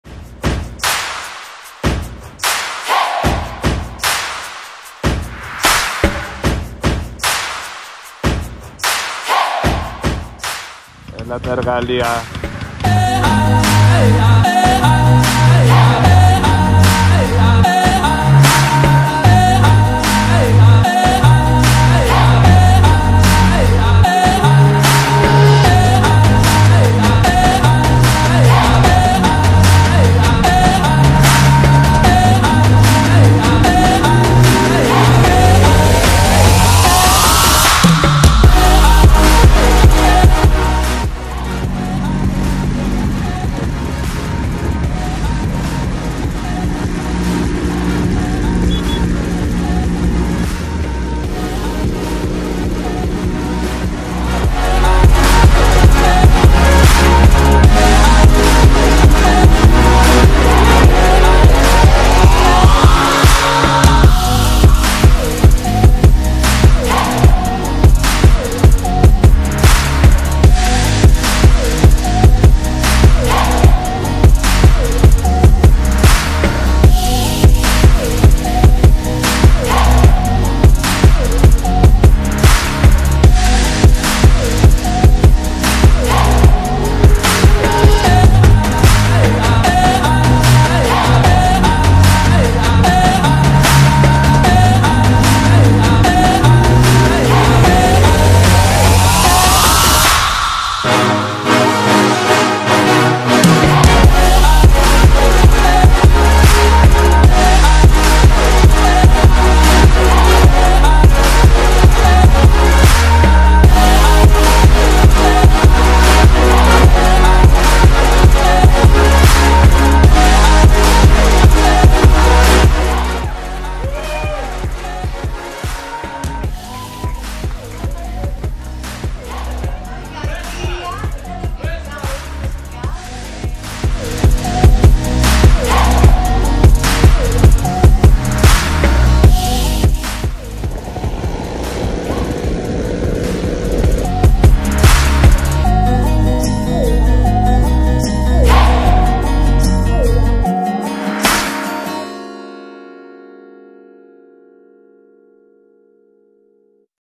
Vesping with friends to Eordaia Tour Vol. I
First Vespa riding in north Greece of the 2023 !